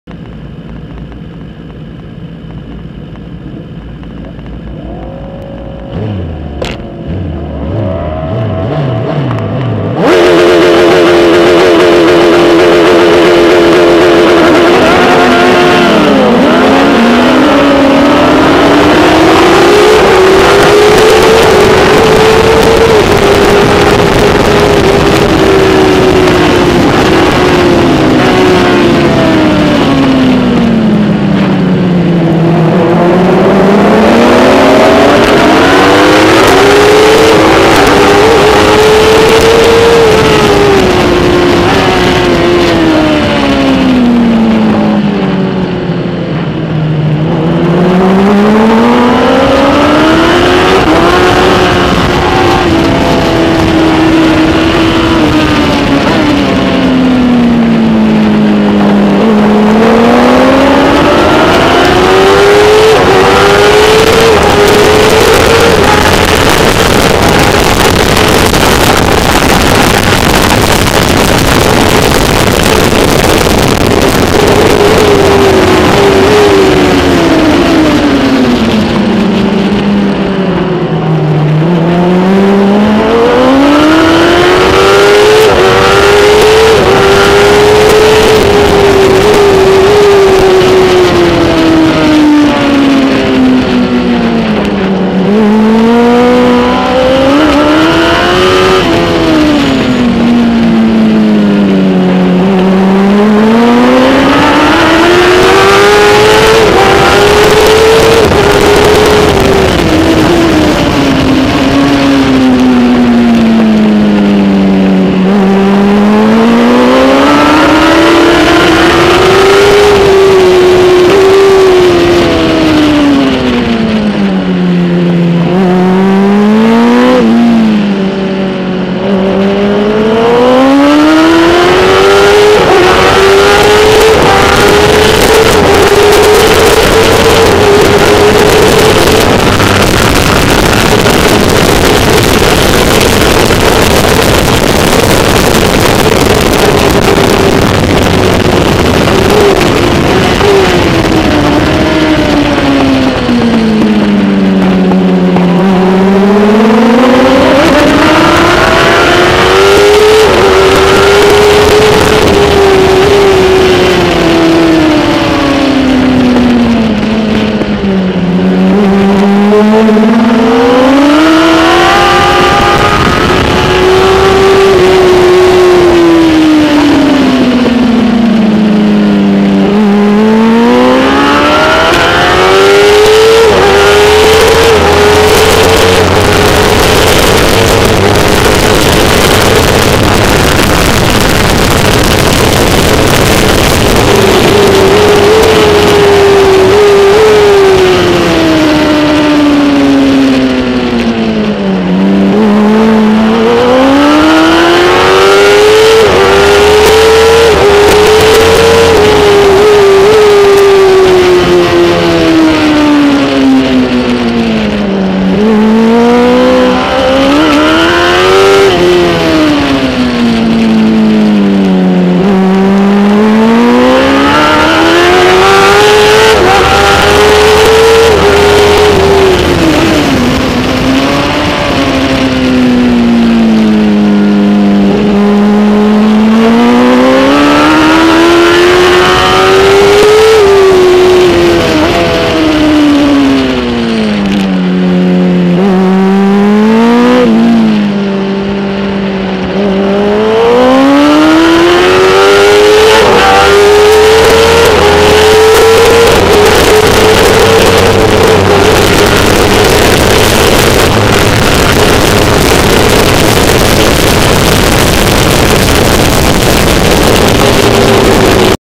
Imatranajo 2017 / IRRC Superbike Race 2 / Onboard Nadieh Schoots
Onboard with Nadieh Schoots on her Kawasaki ZX10R at Imatra (Finland), 9 July 2017, for the start and first two laps of the second IRRC Superbike race.